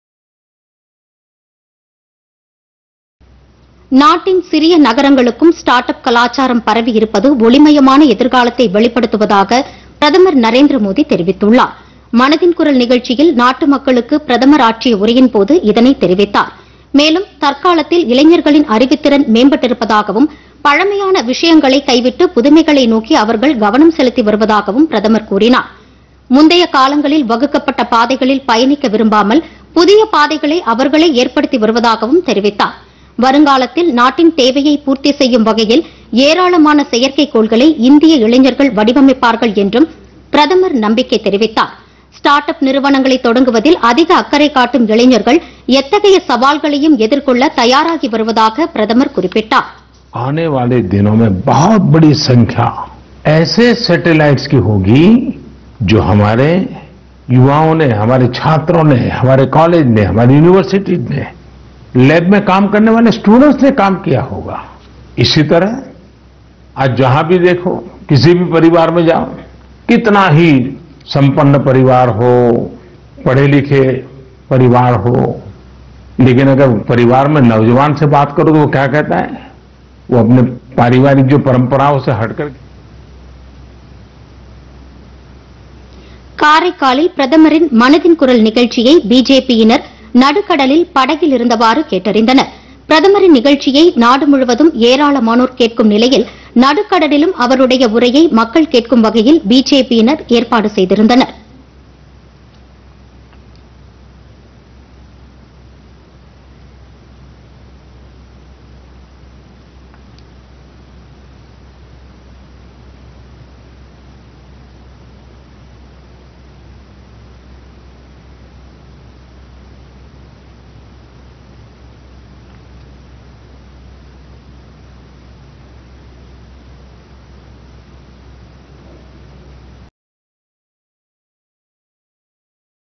ஒளிமயமான எதிர்காலம் உள்ளதுPodhigaiTamilNews
நாட்டின் சிறிய நகரங்களுக்கும், ஸ்டார்ட் அப் கலாச்சாரம் பரவியிருப்பது ஒளிமயமான எதிர்காலத்தை வெளிப்படுத்துகிறது - பிரதமர் நரேந்திர மோதி.